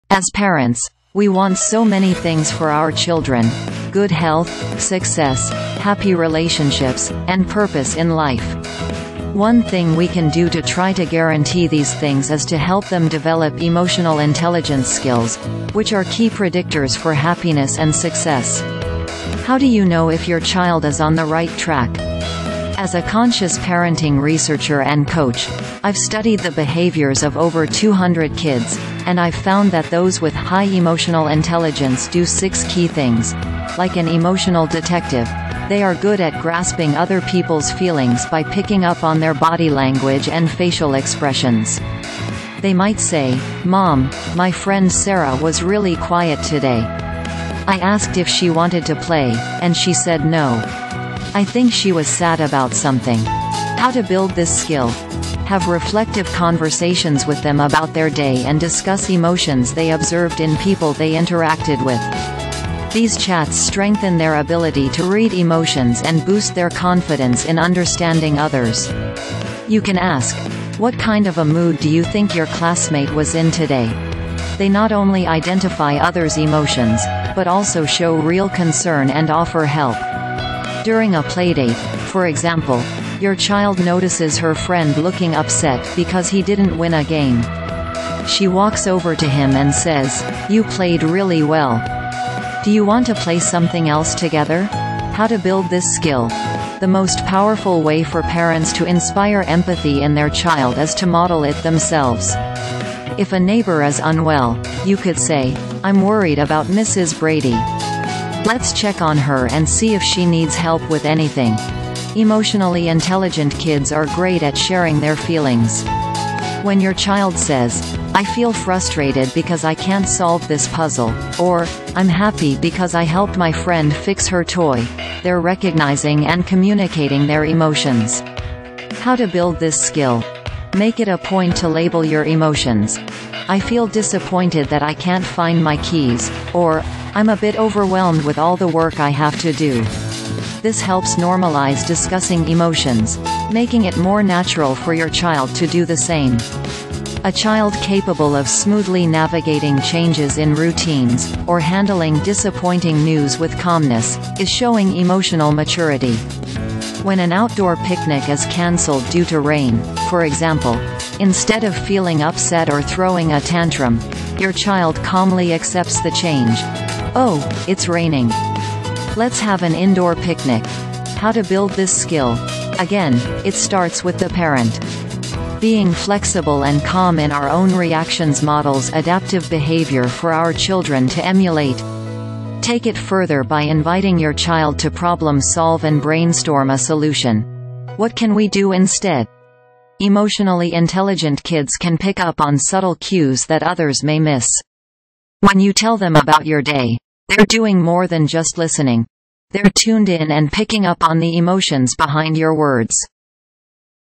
Kids who do these 6 things have 'high emotional intelligence' says
As parents, we want so many things for our children — good health, success, happy relationships, and purpose in life. One thing we can do to try to guarantee these things is to help them develop emotional intelligence skills, which are key predictors for happiness and success. How do you know if your child is on the right track? As a conscious parenting researcher and coach, I've studied the behaviors of over 200 kids, and I've found that those with high emotional intelligence do six key things:Like an emotional detective, they are good at grasping other people's feelings by picking up on their body language and facial expressions. They might say, "Mom, my friend Sarah was really quiet today. I asked if she wanted to play, and she said no. I think she was sad about something."How to build this skill: Have reflective conversations with them about their day and discuss emotions they observed in people they interacted with. These chats strengthen their ability to read emotions and boost their confidence in understanding others. You can ask, "What kind of a mood do you think your classmate was in today?"They not only identify others' emotions, but also show real concern and offer help. During a playdate, for example, your child notices her friend looking upset because he didn't win a game. She walks over to him and says, "You played really well! Do you want to play something else together?"How to build this skill: The most powerful way for parents to inspire empathy in their child is to model it themselves. If a neighbor is unwell, you could say, "I'm worried about Mrs. Brady. Let's check on her and see if she needs help with anything."Emotionally intelligent kids are great at sharing their feelings. When your child says, "I feel frustrated because I can't solve this puzzle," or "I'm happy because I helped my friend fix her toy," they're recognizing and communicating their emotions. How to build this skill: Make it a point to label your emotions: "I feel disappointed that I can't find my keys," or "I'm a bit overwhelmed with all the work I have to do."This helps normalize discussing emotions, making it more natural for your child to do the same. A child capable of smoothly navigating changes in routines, or handling disappointing news with calmness, is showing emotional maturity. When an outdoor picnic is cancelled due to rain, for example, instead of feeling upset or throwing a tantrum, your child calmly accepts the change: "Oh, it's raining. Let's have an indoor picnic!"How to build this skill: Again, it starts with the parent. Being flexible and calm in our own reactions models adaptive behavior for our children to emulate. Take it further by inviting your child to problem-solve and brainstorm a solution: "What can we do instead?"Emotionally intelligent kids can pick up on subtle cues that others may miss. When you tell them about your day, they're doing more than just listening; they're tuned in and picking up on the emotions behind your words.